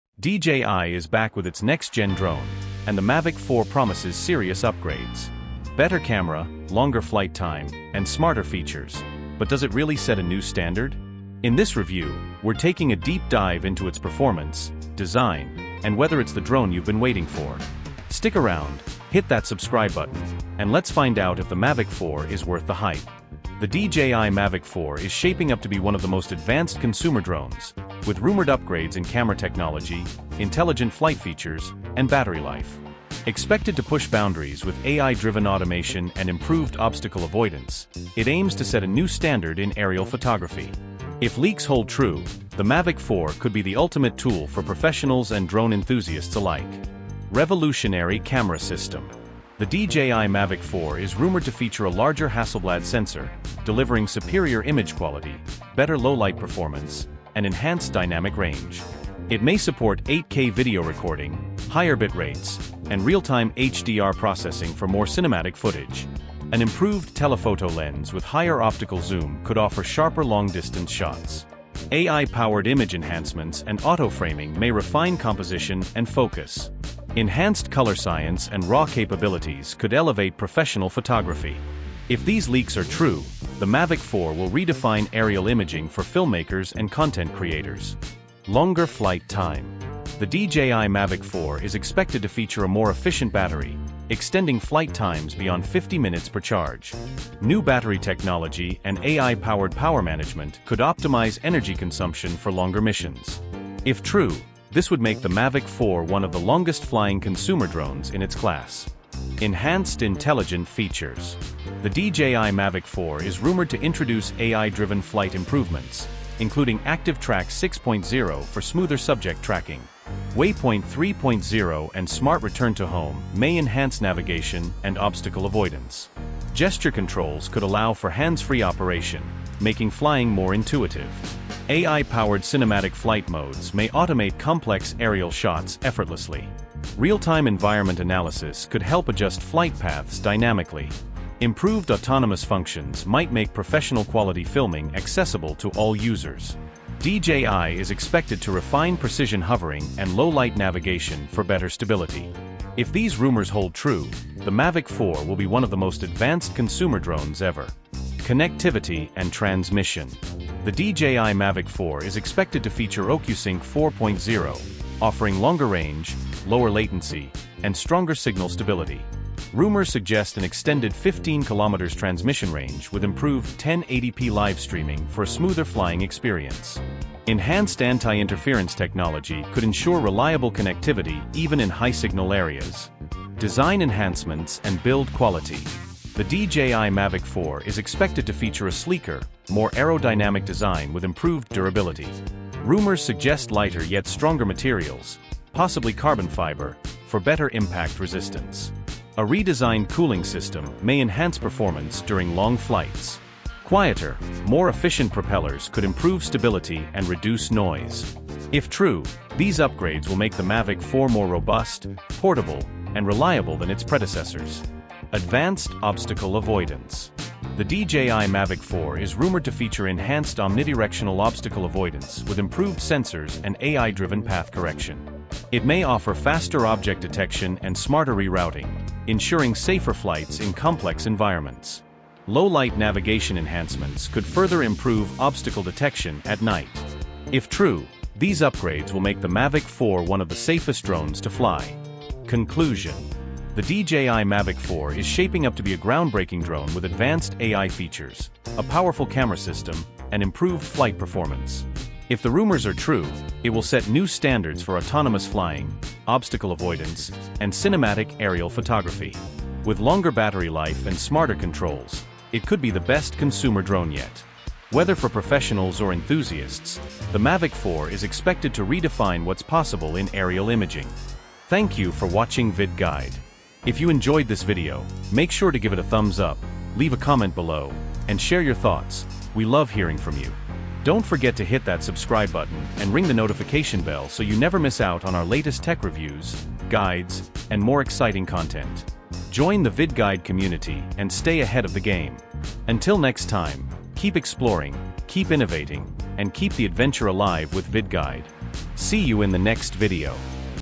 DJI MAVIC 4 PRO Release Date CONFIRMED
The DJI Mavic 4 is rumored to be a game-changer in drone technology, featuring a new Hasselblad camera, 8K video recording, improved low-light performance, and a longer flight time. Leaks suggest an advanced obstacle avoidance system, enhanced AI tracking, and a more compact foldable design for effortless portability. With extended transmission range and smarter flight modes, the Mavic 4 could be the ultimate drone for professionals and enthusiasts alike.

Is the DJI Mavic 4 the best drone upgrade yet? Watch now for leaks, rumored specs, and possible release date!